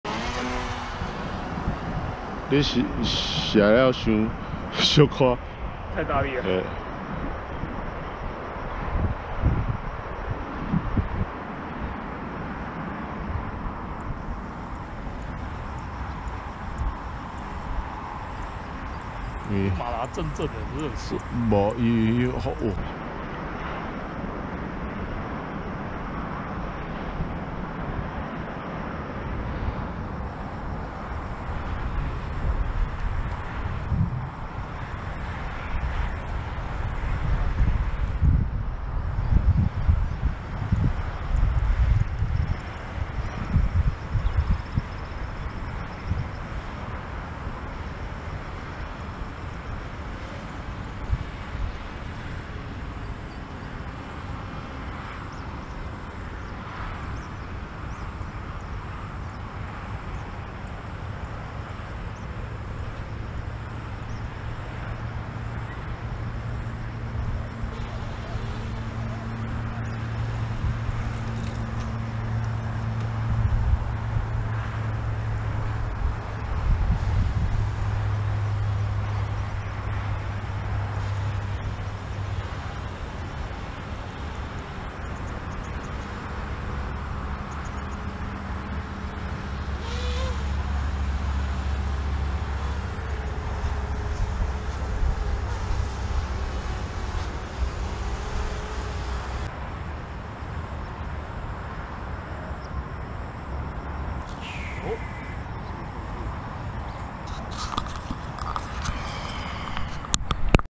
Ranger 1600 flight video
Today is a windy day,
21700 3S1P battery  5000mah
20 min fly, the half throttle, consume power 1593 mah